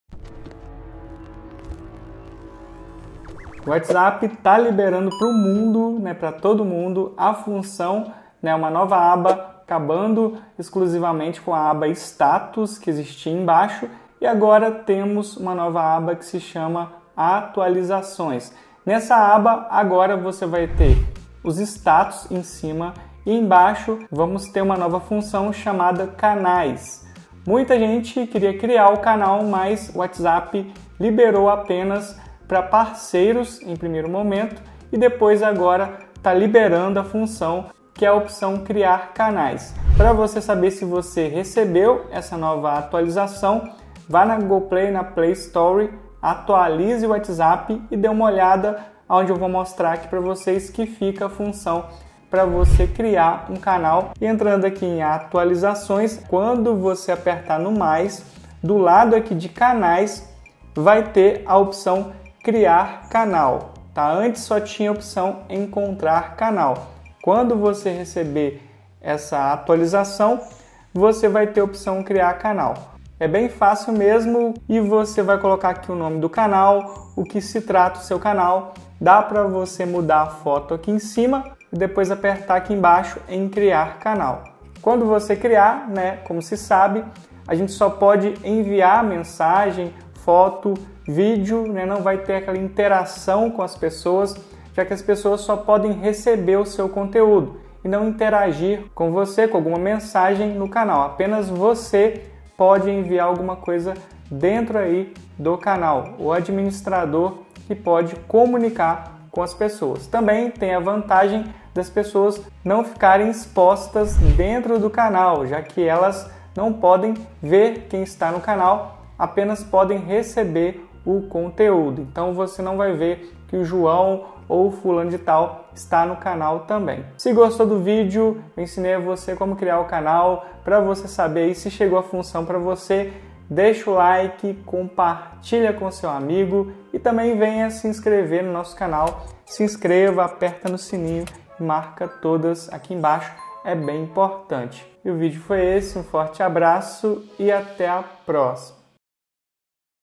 Agora SIM!! Como CRIAR CANAL no WHATSAPP!!
Whatsapp começa a liberar a função de criar um canal no mensageiro e nesse vídeo eu vou ensinar a como criar uma canal no Whatsapp em nosso sistema Android.